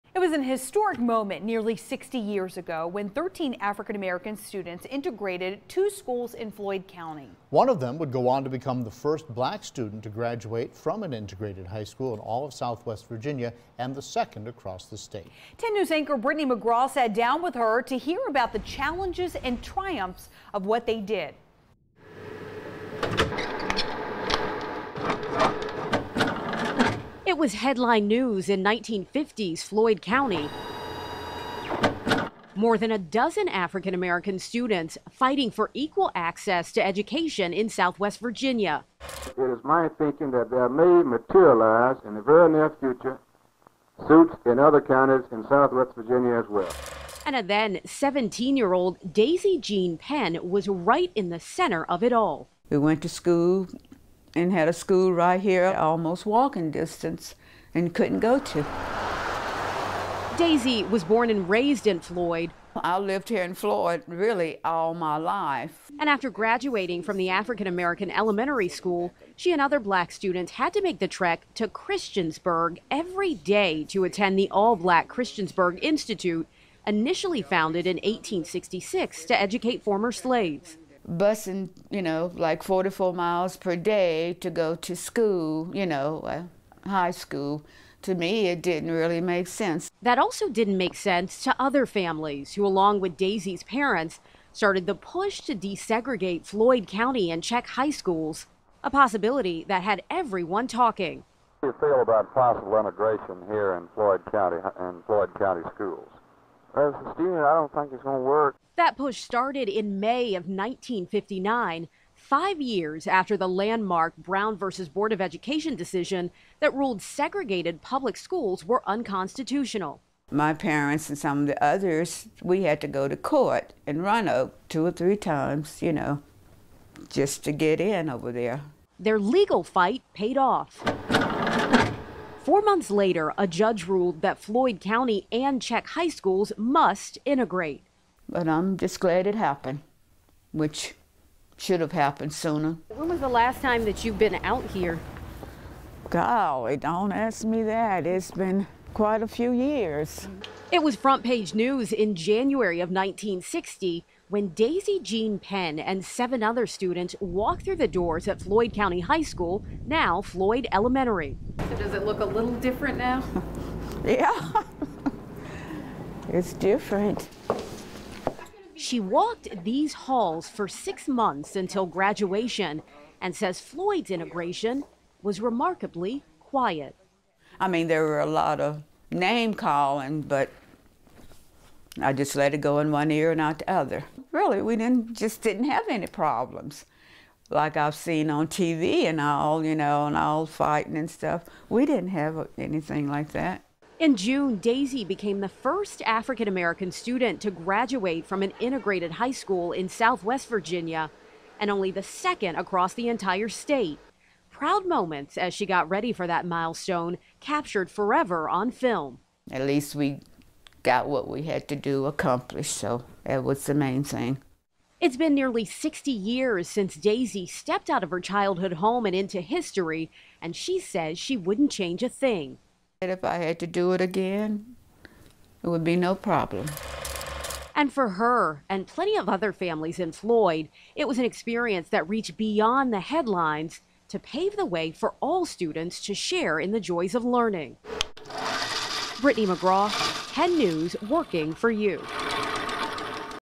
First Black woman to graduate from integrated high school in southwest Virginia speaks to 10 News
Nearly 60 years ago, 13 African American students integrated two schools in Floyd County.